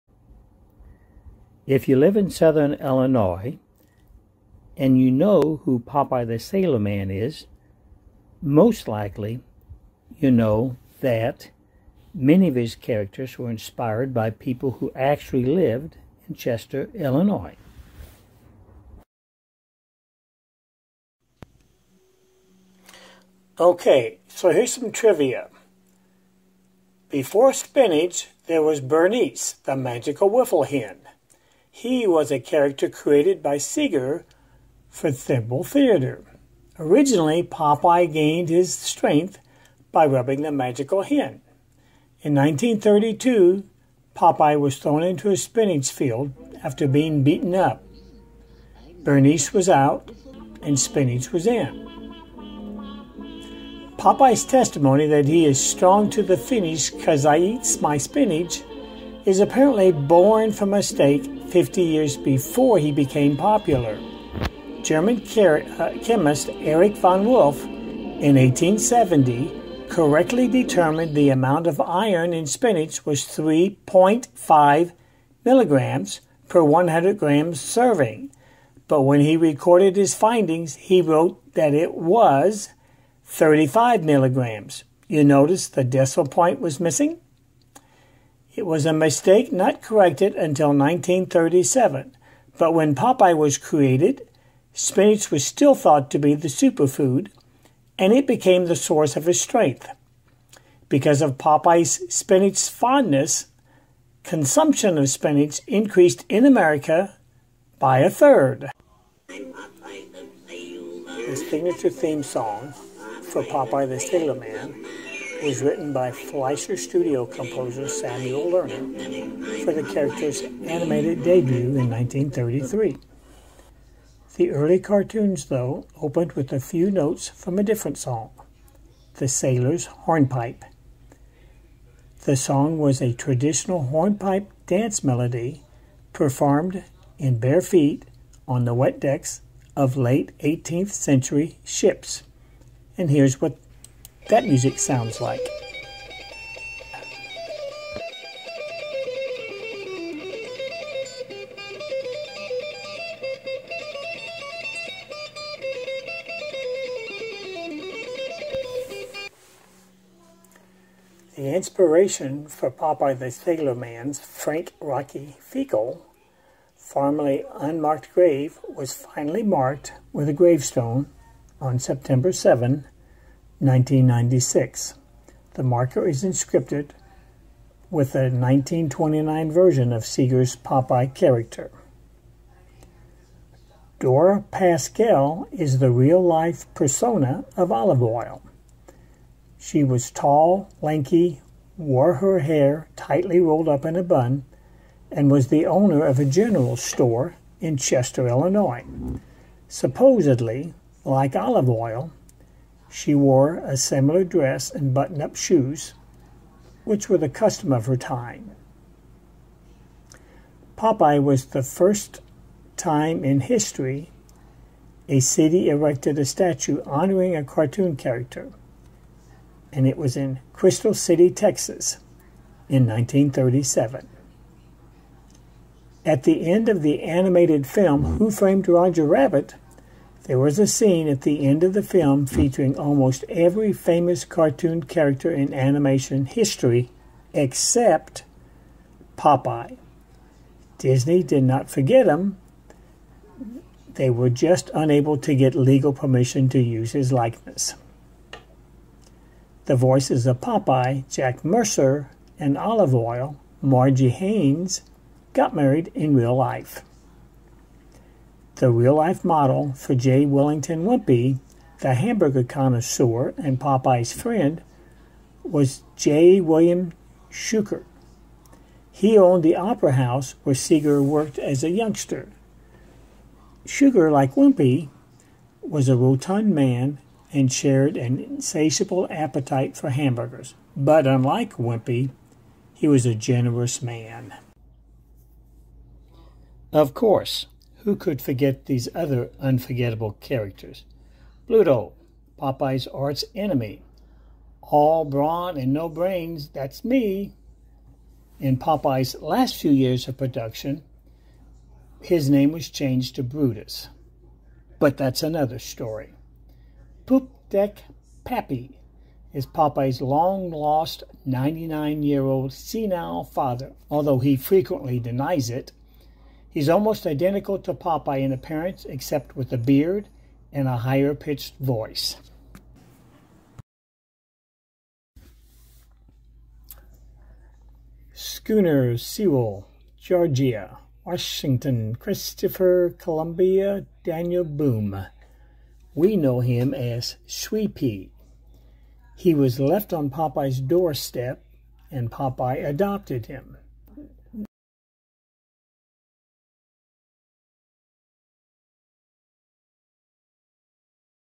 Popeye trivia
Part II of Popeye history. Some trivia.